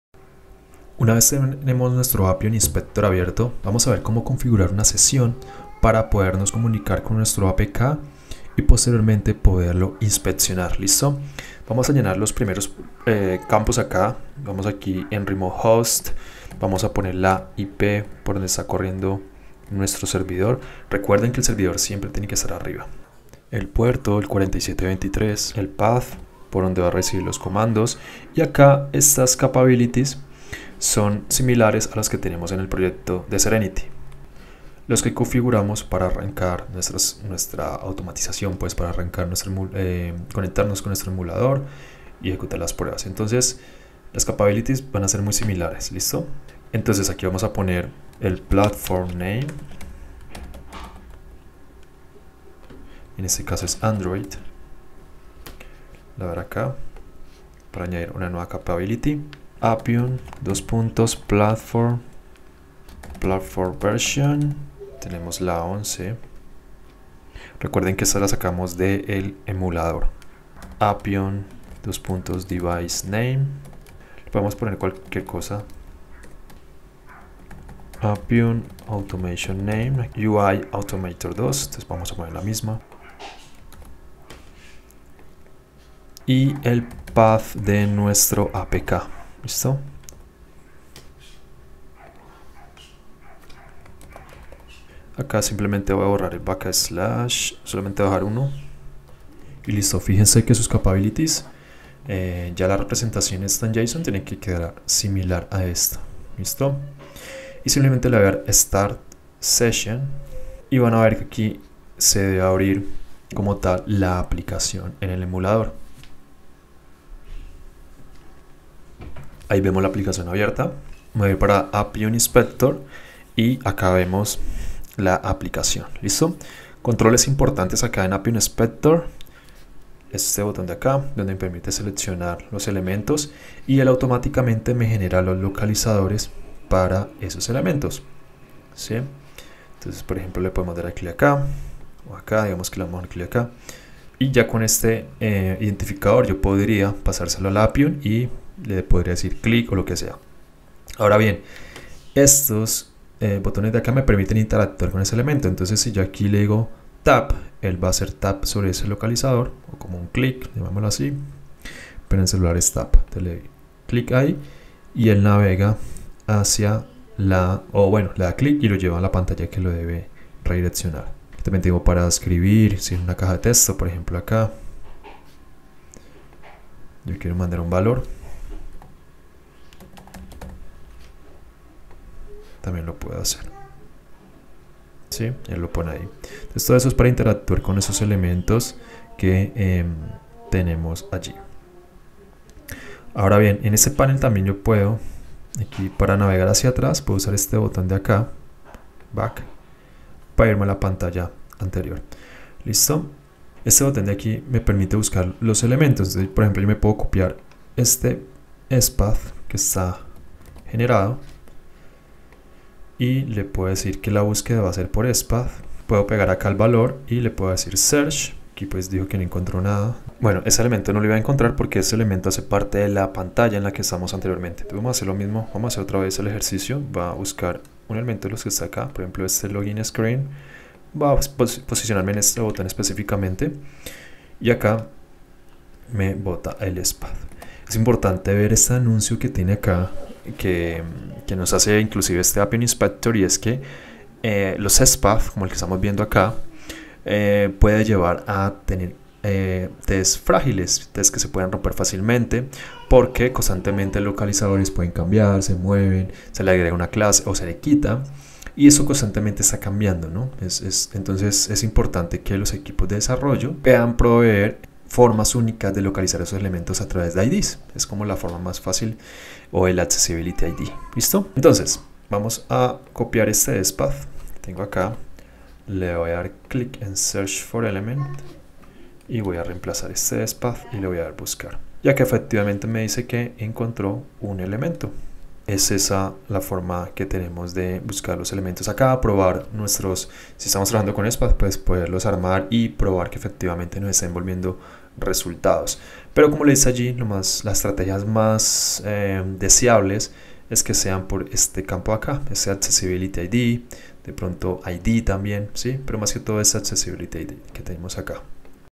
Configuración de Appium Inspector | Automatización con Appium y Serenity BDD | Parte 10
En esta lección vamos a ver como configurar Appium Inspector para analizar nuestra aplicación móvil.

Aquiere el curso de creación de framework de automatización con Selenium y Springboot